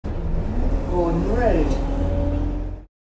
Gordon Road
iBus announcement recorded on route K5 (SDE20283) on the 30/07/2020